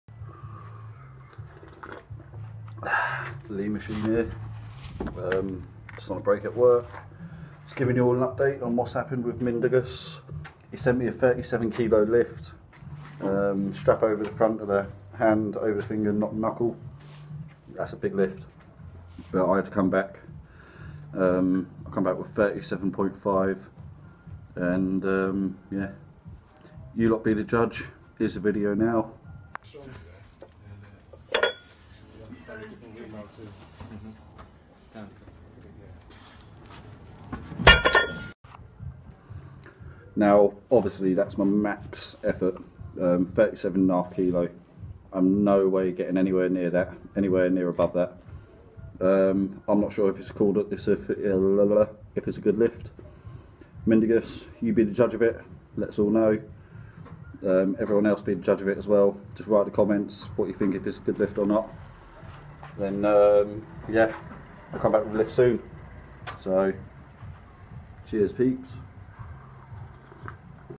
Tom Holland replies to Tarasaitis 37kg
Tom Holland replies to Mindaugas Tarasaitis 37kg riser challenge, was my lift in good enough form to beat him?? 🤷‍♂️